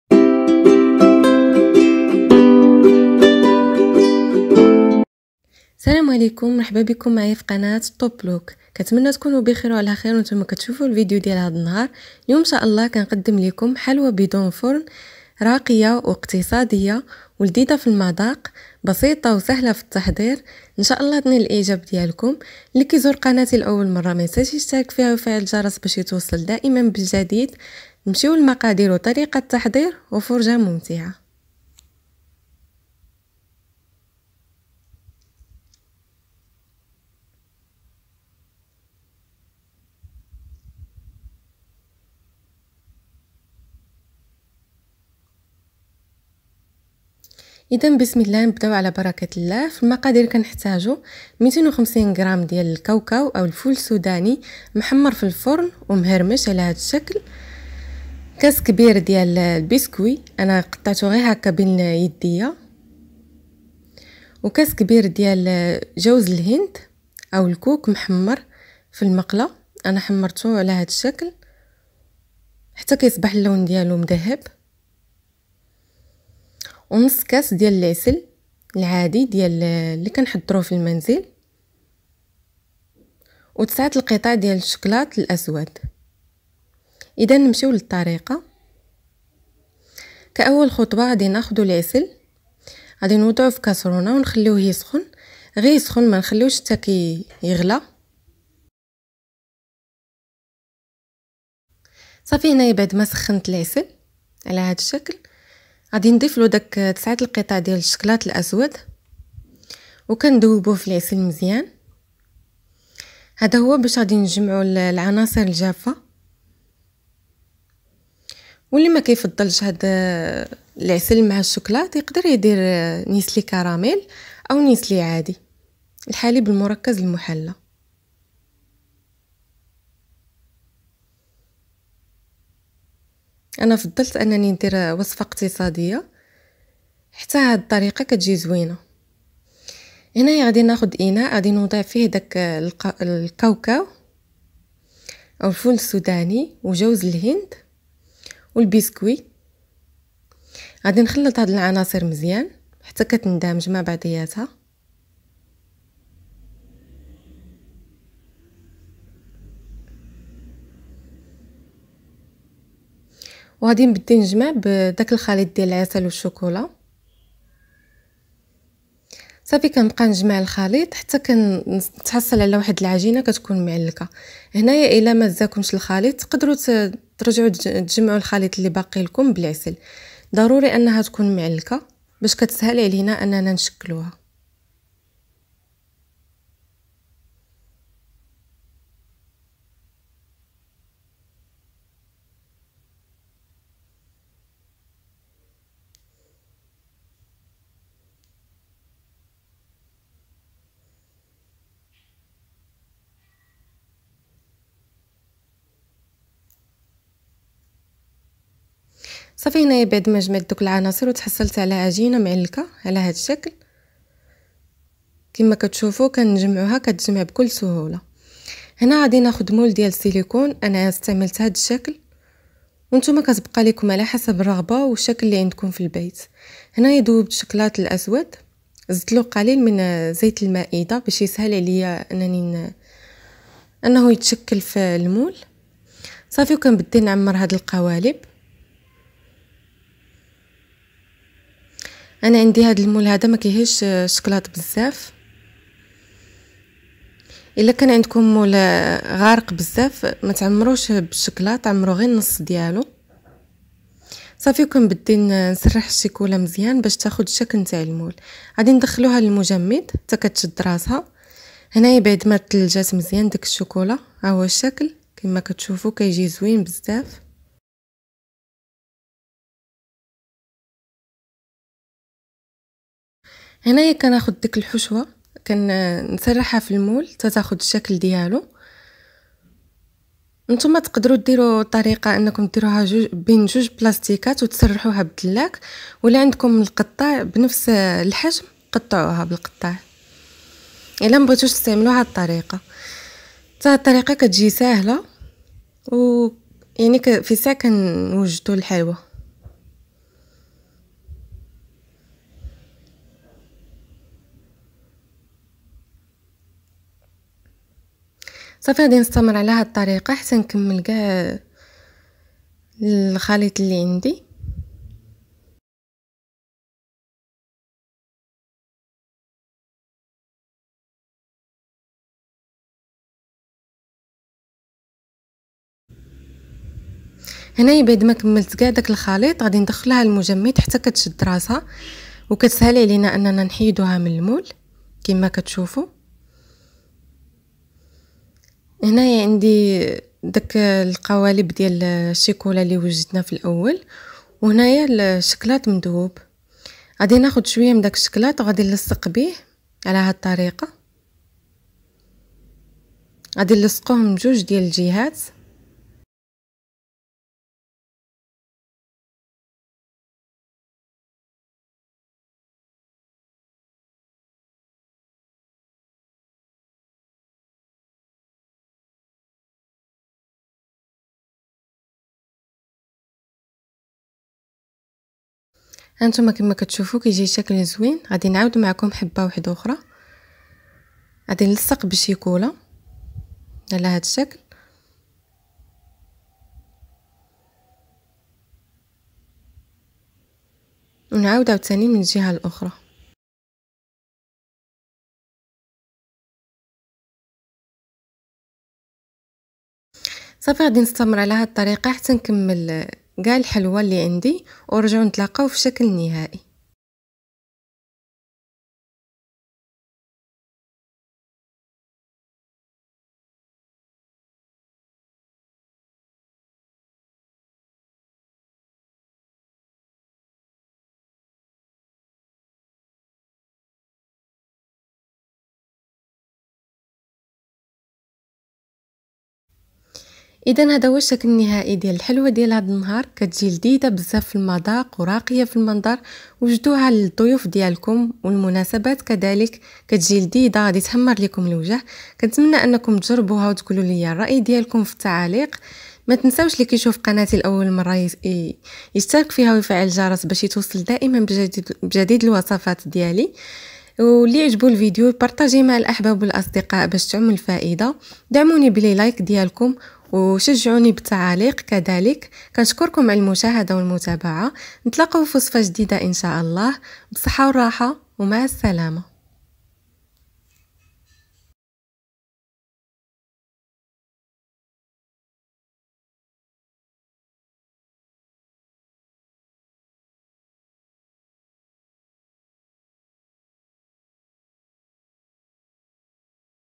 حلوة الشكلاط والكاوكاو في 10 دقائق بدون فرن راقية وبمذاق رائع سهلة التحضير
مرحبا بيكم فقناة طوب لووك
طوب لووك كتقدم ليكم حلوة الشكلاط والكاوكاو بدون فرن سهلة سريعة التحضير وراقية فنفس الوقت من الذ مايكون
ولتساؤلاتكم اتركوا لنا تعليقاتكم وسنجيب عنها بإذن الله..

شكرا لكم بزاف على المشاهدة نتمنى ينال الفيديو اعجابكم
متنساوش تشتاركوا فالقناة وتفعلوا الجرس باش توصلوا بجديد الوصفات
موعدنا فالفيديو القادم ان شاء الله